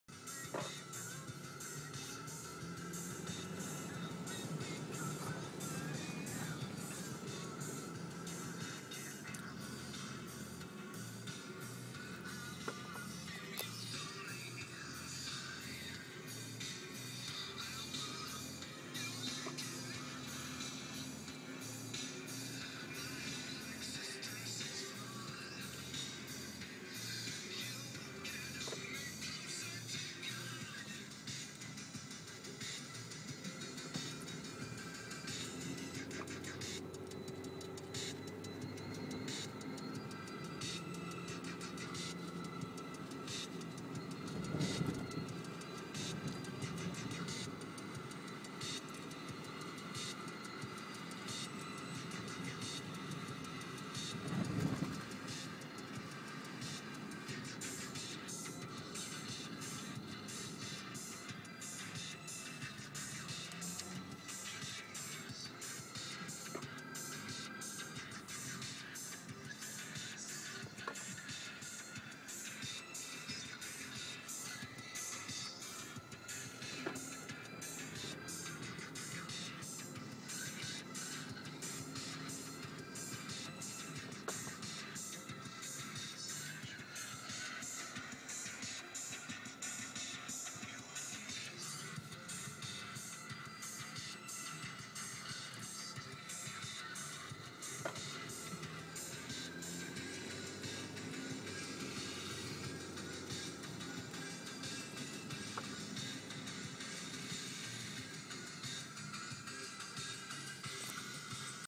Flood: March 7th 2011 (Pompton Lakes)
Driving around the flood prone streets of Pomton Lakes and Wayne New Jersey.